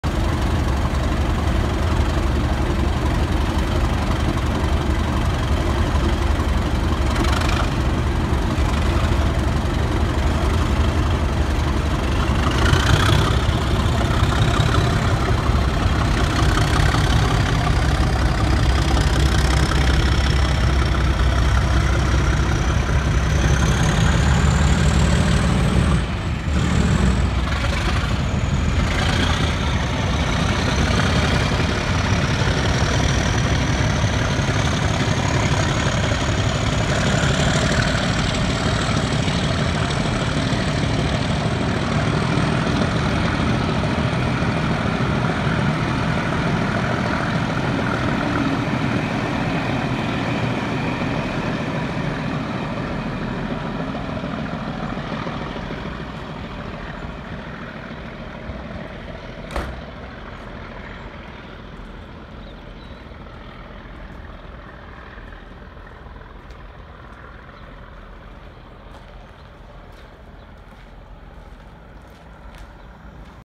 37403 and 37685 depart Ladybank 1Z37
SRPS railtour to Aviemore and the Strathspey Railway